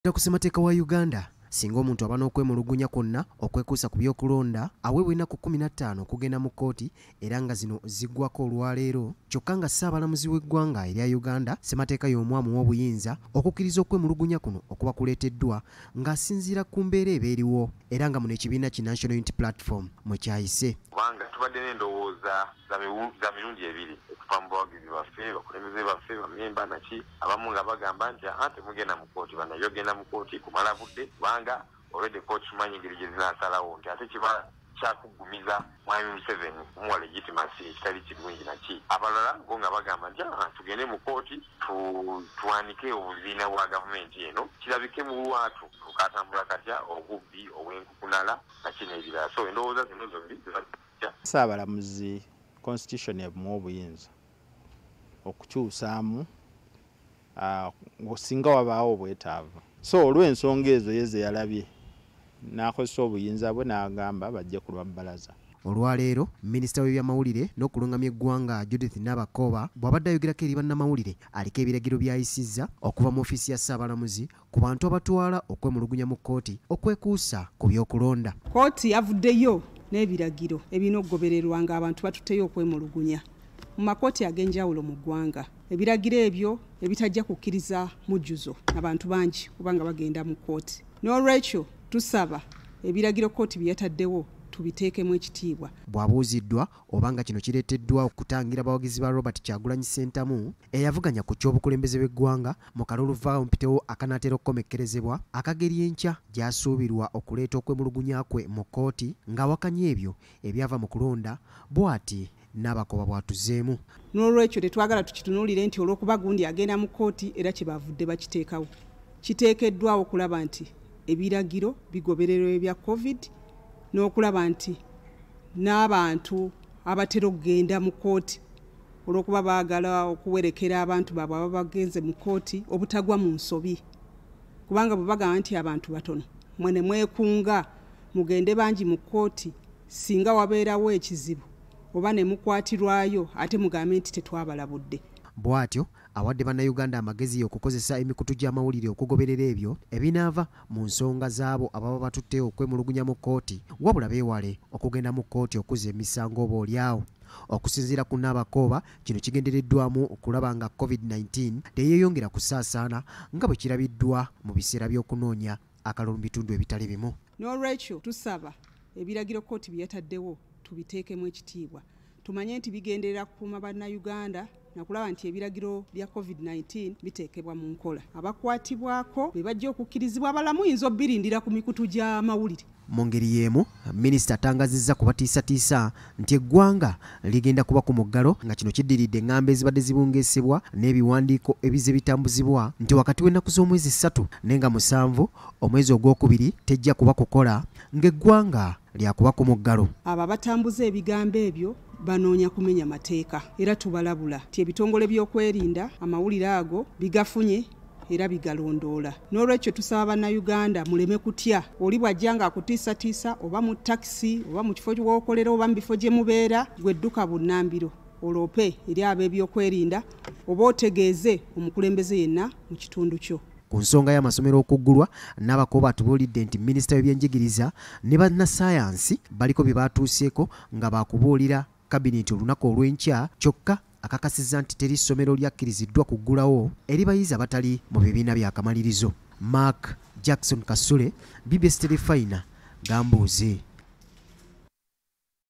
Gavumenti Ereese Ebiragiro Ku Batwala Emisango Gy’okulonda Mu Kkooti - Minisita Nabakooba
Gavumenti eriko ebiragiro byeyisizza ku bantu abalina okuba ku kkkooti naddala mu kaseera kano nga abeemulugunya ku byava mu kalulu beyongera.

Okusinziira ku kiwandiiko kyasomye okuva eri Ssaabalamuzi wa Uganda, Minisita w'Ebyamawulire nokulungamya eggwanga Judith Nabakooba atubuulidde nti kino kikoleddwa mu mutima gwakuziyiza kusasaana kwa Covid-19.

Kino wekijidde n’gabantu abaagala okutwala okwemulugunya mu kkooti beyongera buli lukya.